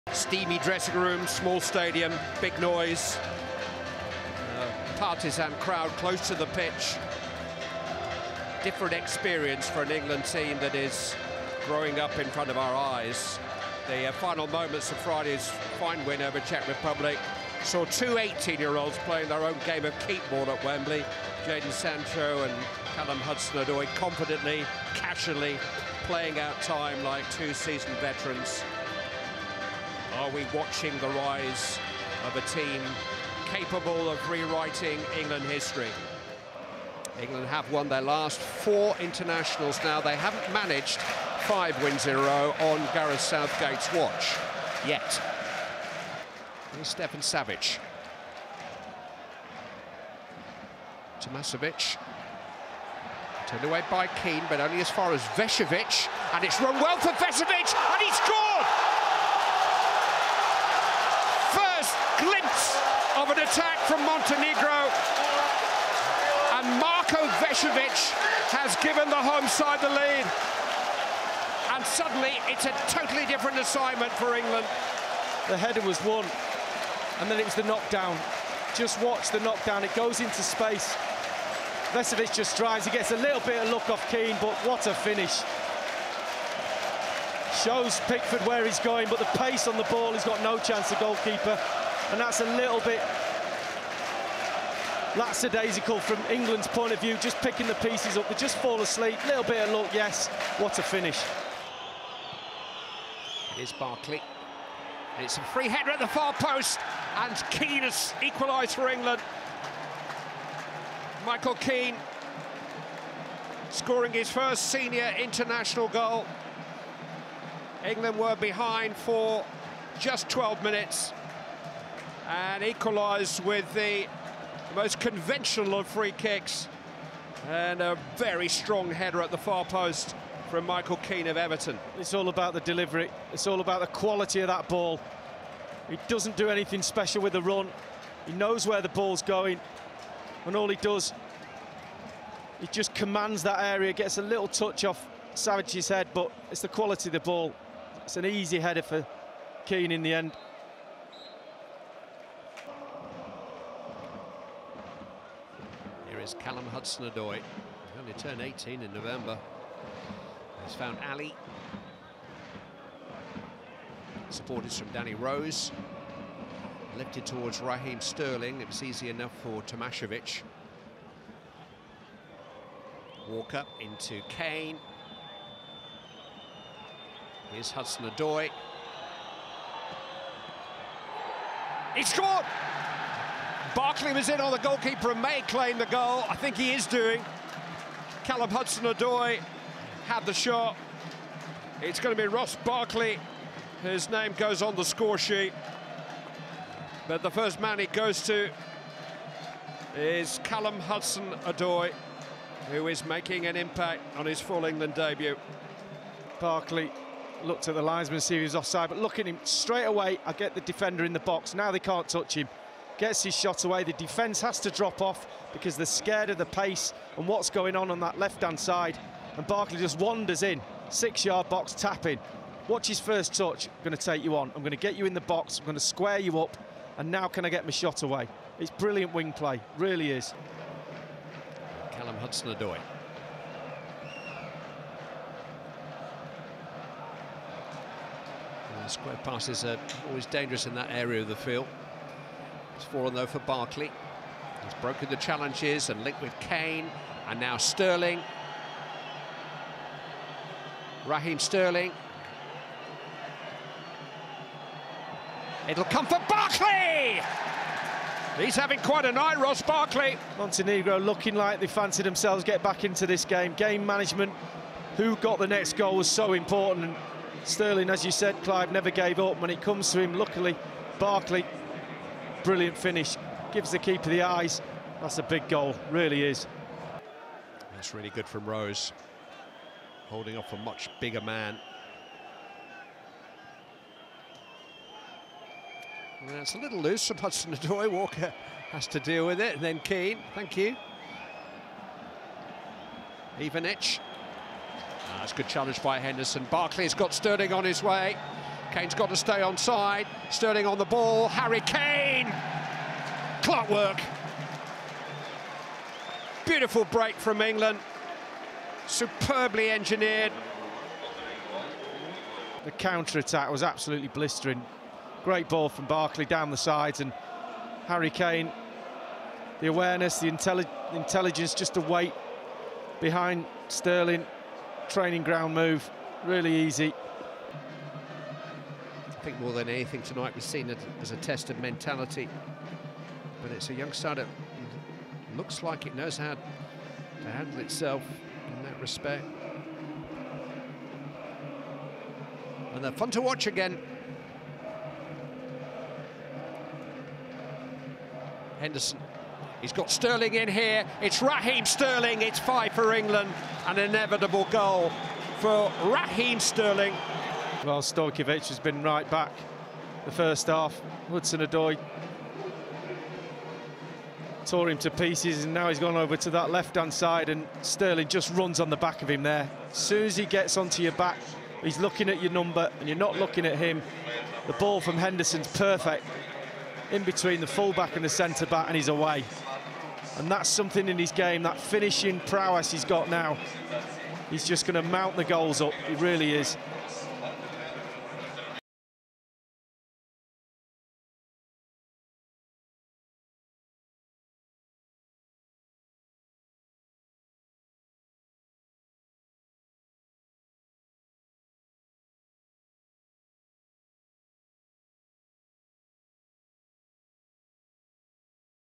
Montenegro 1-5 England | Barkley Brace & Sterling Shines Again! | Euro 2020 Qualifiers
Raheem Sterling shines again as England secure their second Euro 2020 Qualifiers win of the campaign, beating Montenegro 5-1 – with goals from Ross Barkley, Michael Keane, Harry Kane, and Raheem Sterling.